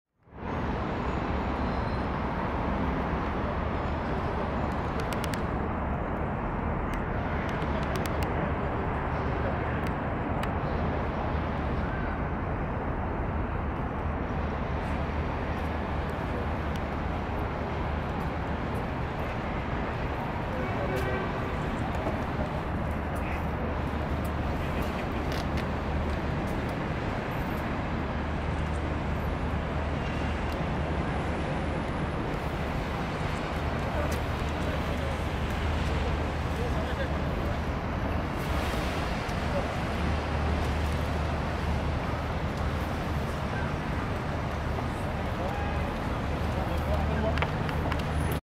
Pont des Arts
Un touriste vandale pose un cadenas en risquant sa vie.